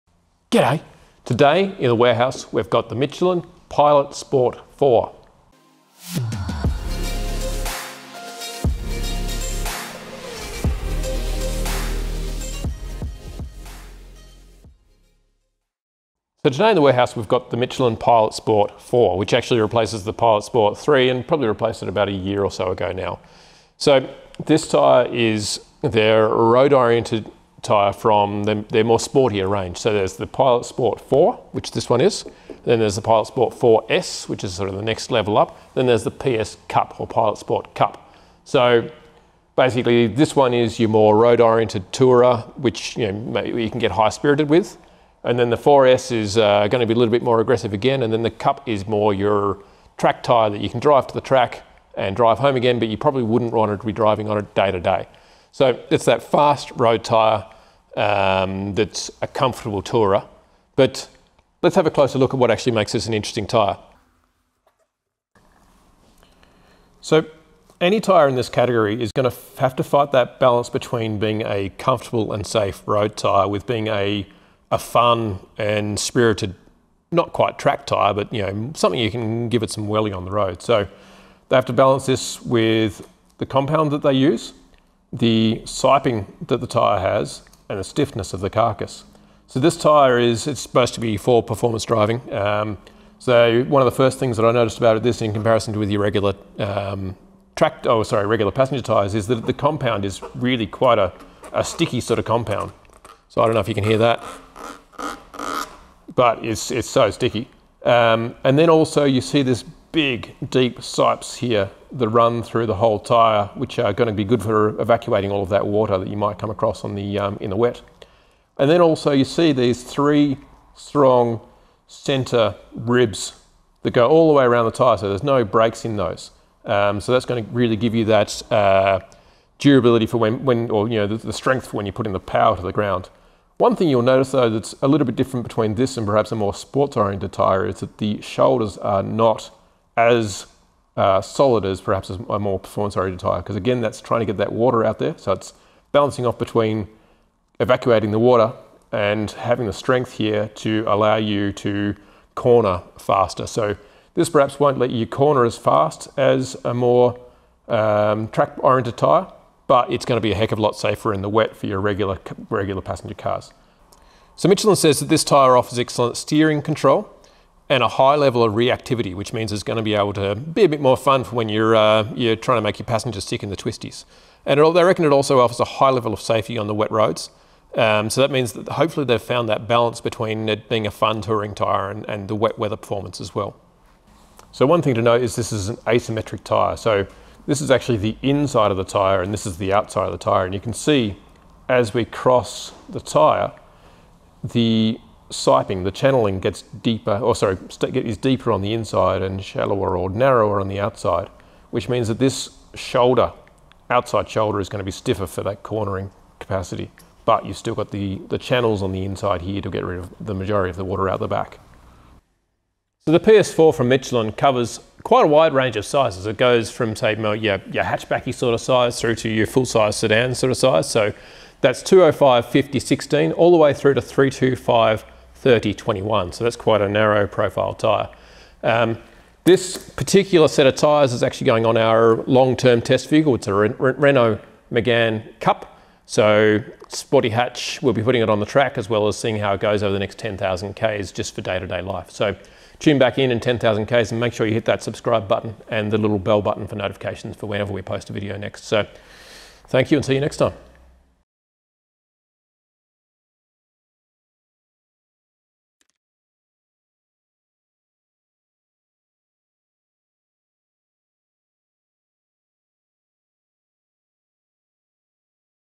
Michelin Pilot Sport 4 Studio Overview
We're taking a look at the Michelin Pilot Sport 4, which is a replacement for the Michelin Pilot Sport 3. The Pilot Sport 4 tyre is a mix between a touring tyre with a dash of performance driving thrown in. Let's have closer look!

Full Transcript:

- G'day. Today, in our warehouse, we've got the Michelin Pilot Sport 4. So today in the warehouse, we've got the Michelin Pilot Sport 4, which actually replaces the Pilot Sport 3, and probably replaced it about a year and so ago now. So, this tyre is their road oriented tyre, from their more sportier range. So there's the Pilot Sport 4, which this one is, and there's the Pilot Sport 4S, which is sort of the next level up, then there's the PS Cup or Pilot Sport Cup.

So, basically this one is your more road oriented tourer which maybe you can get high-spirited with. And then the 4S is going to be a little bit more aggressive again and then the Cup is more your track tyre that you can drive to track and drive home again but you probably wouldn't want to be driving on a day-to-day. So it's that fast road tyre that's a comfortable tourer, but have let's a closer look at what actually makes this an interesting tyre. So, any tyre in this category is going to have to fight that balance between being a comfortable and safe road tyre with being a fun and spirited, not quite track tyre, but something you can give it some whirling on the road.

So they have to balance this with the compound that they use, the siping that the tyre has, and the stiffness of the carcass. So this tyre is supposed to be for performance driving, so one of the first that I noticed about it did seem, in comparison with your regular passenger tyres is that the compound is really quite a sticky sort of compound. So I don't know if you can hear that, but it's so sticky. And then also, you see these big, deep sipes here that run through the whole tyre, which are going to be good for evacuating all of that water that you might come across in the wet. And then also, you see these three strong stinter ribs? They go all the way around the tyre, so there's no breaks in those.
So that's going to really give you that durability for when, or the strength for when you're putting the pedal to the ground. One thing you'll notice, though, that's a little bit different between this and perhaps a more sport oriented tyre, it's that the shoulders are not as solid as perhaps a more performance oriented tyre. Because again, that's trying to get that water out there, so it's balancing off between evacuating the water and having the strength here to allow you to corner faster. So this perhaps won't let you corner as fast as a more track oriented tyre, but it's going to be a heck of a lot safer in the wet for your regular passenger cars.

So Michelin says that this tyre offers excellent steering control and a high level of reactivity, which means it's going to be able to be a bit more fun for when you're trying to make your passengers take in the twisties. And they also reckon it offers a high level of safety on the wet roads, so that means that hopefully they found that balance between it being a fun touring tyre and the wet weather performance as well. So one thing to know is this is an asymmetric tyre.

So this is actually the inside of the tyre, and this is the outside of the tyre, and you can see, as we cross the tyre, the siping, the channelling, gets deeper also, it's deeper on the inside and shallower or narrower on the outside, which means that this shoulder, outside shoulder is going to be stiffer for that cornering capacity. But you've still got the channels on the inside here to get rid of the majority of the water out the back.

So the PS 4 from Michelin covers quite a wide range of sizes. It goes from say, your hatchback sort of size through to your full-size sedan sort of size, so that's 205/50-16 all the way through to 325/30-21, so that's quite a narrow profile tyre. This particular set of tyres is actually going on our long-term test vehicle, it's a Renault Megane Cup, so sporty hatch. We'll be putting it on the track, as well as seeing how it goes over the next 10,000 kms just for day-to-day life.